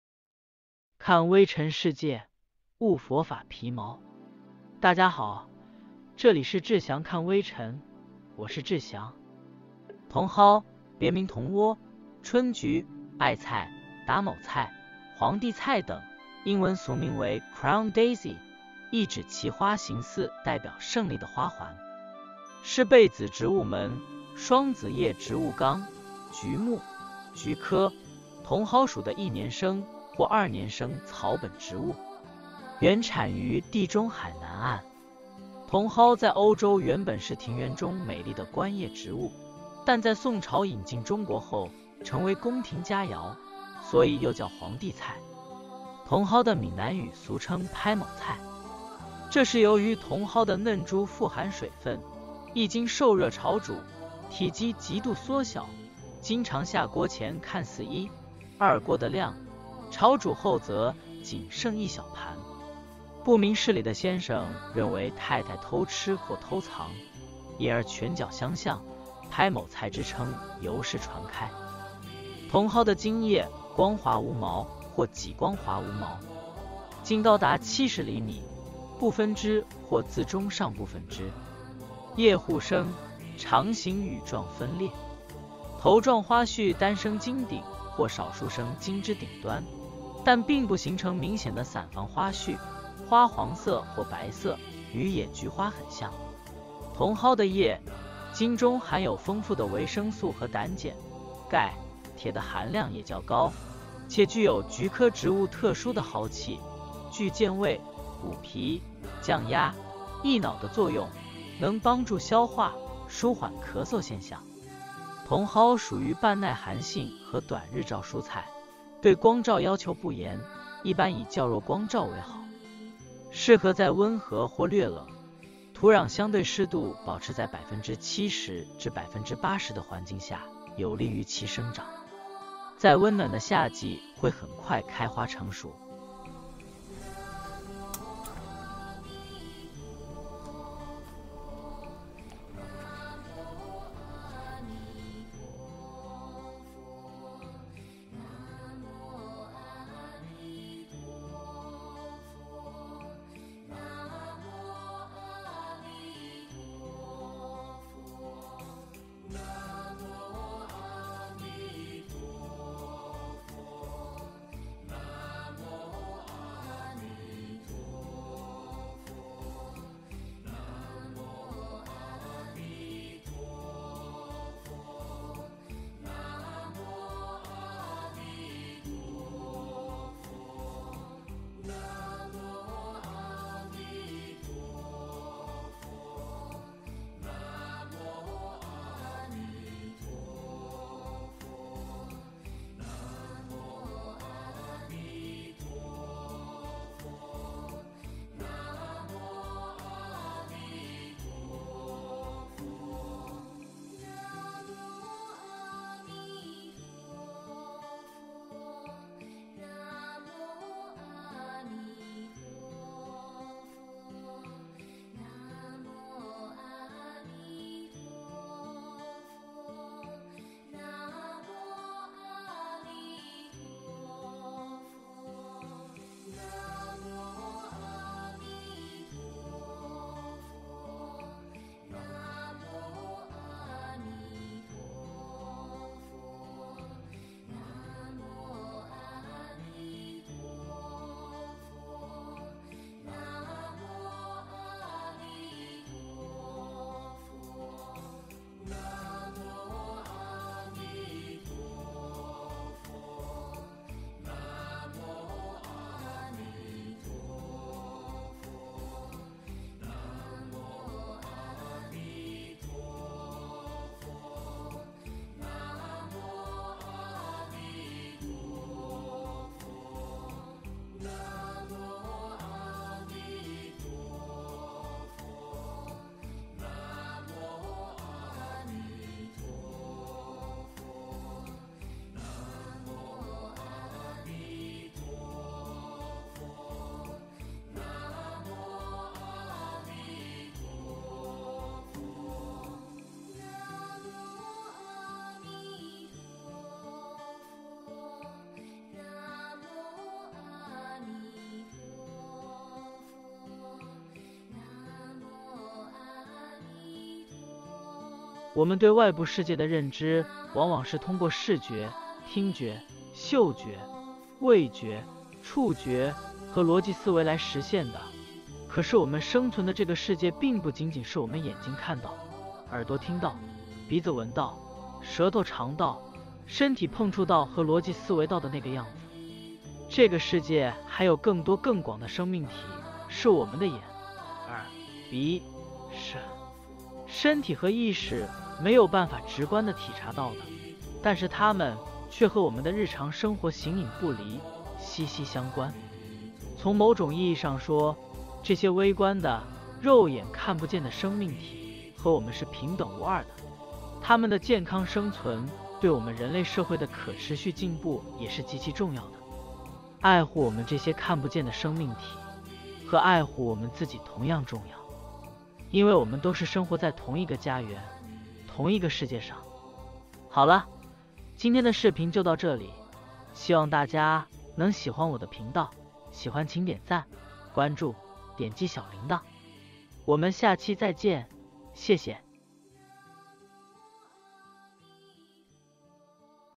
菜園子系類2：歷史上只有皇帝才能吃到的宮廷佳餚——茼蒿
茼蒿，別名茼莴、春菊、艾菜、打某菜、皇帝菜等，英文俗名为Crown Daisy，意指其花形似代表胜利的花环。是被子植物门、双子叶植物纲、菊目、菊科、茼蒿属的一年生或二年生草本植物。原产于地中海南岸。茼蒿在欧洲原本是庭园中美丽的观叶植物，但在宋朝引进中国后，成为宫廷佳肴，所以又叫皇帝菜。茼蒿的闽南语俗称拍某菜。这是由于茼蒿的嫩株富含水分，一经受热炒煮，体积极度缩小，经常下锅前看似一、二锅的量，炒煮后则仅剩一小盘。不明事理的先生认为太太偷吃或偷藏，因而拳脚相向，拍某菜之称由是传开。

茼蒿的茎叶光滑无毛或几光滑无毛。茎高达70厘米，不分枝或自中上部分枝。叶互生，长形羽状分裂。头状花序单生茎顶或少数生茎枝顶端，但并不形成明显的伞房花序，花黄色或白色，与野菊花很像。

茼蒿的叶、茎中含有丰富的维生素和胆碱，钙、铁的含量也较高，且具有菊科植物特殊的蒿气，具健胃、补脾、降压、益脑的作用，能帮助消化、舒缓咳嗽现象。

茼蒿属于半耐寒性和短日照蔬菜，对光照要求不严，一般以较弱光照为好。适合在温和或略冷、土壤相对湿度保持在70%~80%的环境下有利于其生长。在温暖的夏季会很快开花成熟。种子于初春或秋季播撒。

若有见闻者，悉发菩提心，